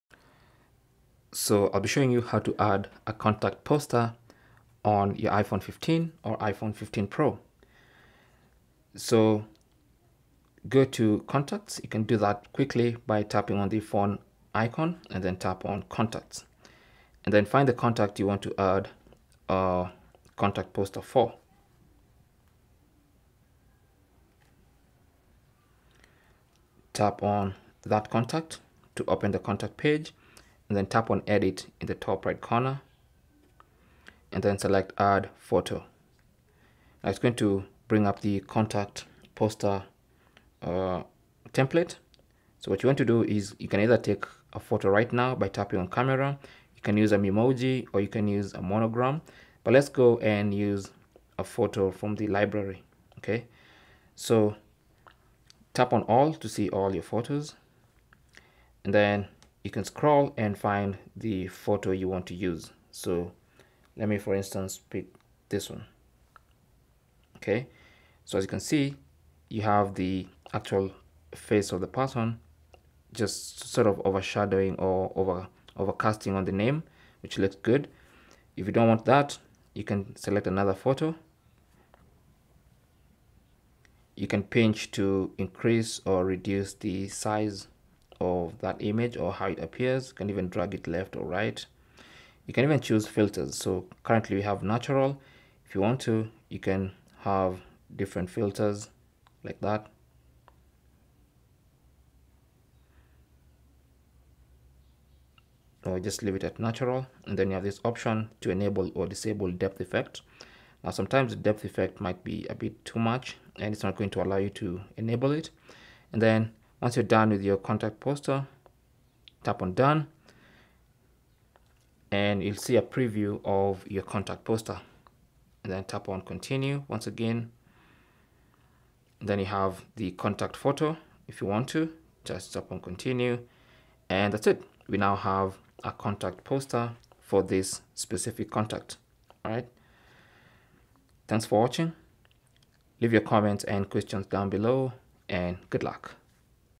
How To Add Contact Poster On iPhone 15 & iPhone 15 Pro
In this step-by-step tutorial, we'll show you exactly how to add a contact poster on your iPhone 15 and iPhone 15 Pro, to make your interactions more visually appealing and easily identifiable.

These instructions will apply to the iPhone 15 Pro Max as well. This is what has been used for this demonstration.

Add Contact Poster On iPhone 15 & iPhone 15 Pro:

Step 1: Locate the Contacts app on your iPhone's home screen and tap to open it. You can also access the contacts via the Phone app, in the "Contacts" tab.

Step 3: Tap on "Edit" at the top right corner, then select 'Add photo' to access the Contact poster templates on your iPhone.

Step 4: On the Contact poster template, look through the options and find the one you'd prefer. You have the following options to choose from:

Camera: Take a photo with the Camera app.
Photos: Choose an already existing photo from the Photos library.
Memoji: Select a Memoji for the contact from a catalogue of Memoji.
Monogram: Choose an alphabet lettered monogram from a selection of them.

Once you choose the option you want, say, an existing photo, you can now make adjustments to it.

Step 5: At this stage, you can customize the appearance of the contact poster. You'll have options to pinch to crop, filters to use and enable or disable depth effect. Make your preferred selections to personalize the contact poster.

Step 6: Once you're satisfied with the customization, tap on the "Done" button to save the contact poster to your device. After saving, you'll be able to see a preview of what it looks like. Tap on "Continue" and this will be applied to that particular contact.

Step 7: You can decide to add a contact photo if you'd like. To do so, tap on 'Choose a Different Photo' and follow any on-screen instructions that will appear or simply tap on "Continue" once again to use the one from the contact poster. If you'd like to have the poster alone, you can tap on 'Skip'.

Step 8: Finally, in the selected contact's page, tap on "Done" to save the changes you have made.

WOTOBEUS USB-C to USB-C Cable 5A PD100W Cord LED Display:

Cell Phone Tripod Adapter